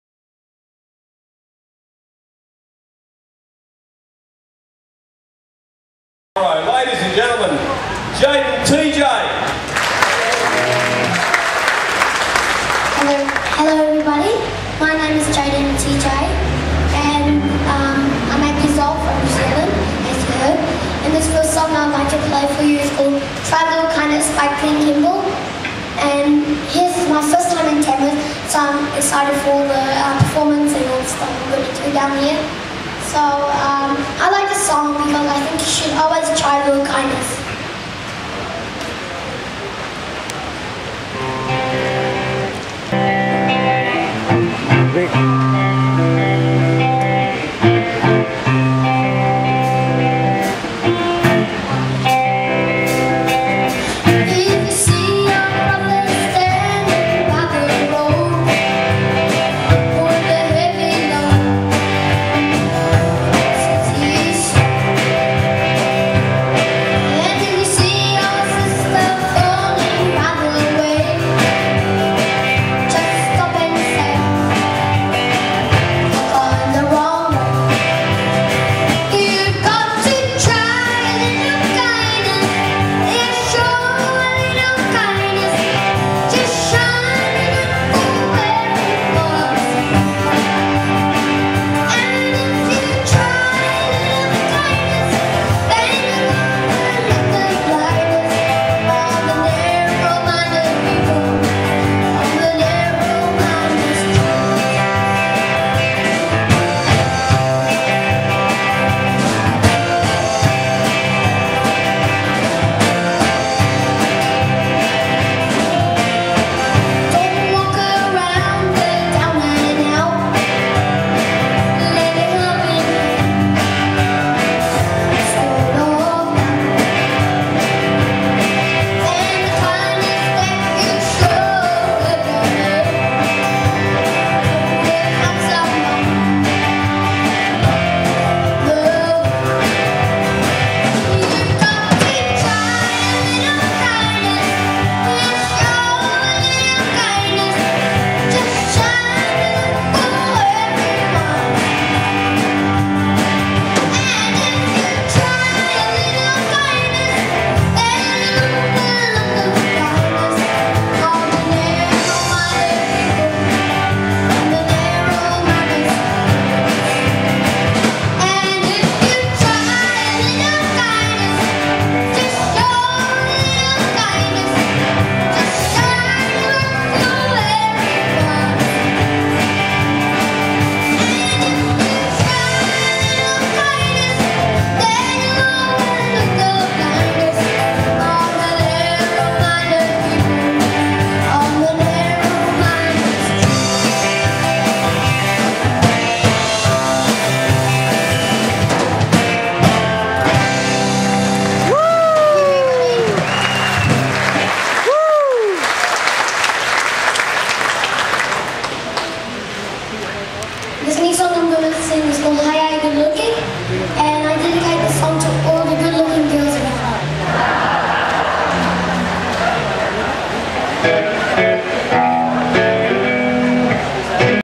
Jayden TJ: Winner of 2013 Battle of Young Stars (Junior)- Tamworth CMF
This is Jayden TJ (8 years old), winner of the 2013 Battle of the Young Stars in Tamworth Country Music Festival, Australia.